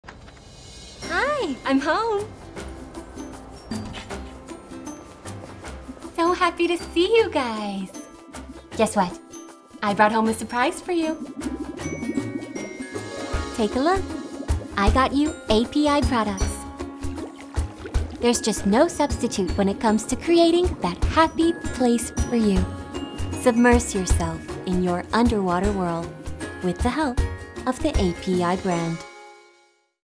API Brand Commercial (USA)
API brand is passionate about fishkeeping. We’ve been a pioneer in the aquatics industry for almost 60 years, developing and manufacturing hundreds of quality products to help people create and enjoy their underwater worlds.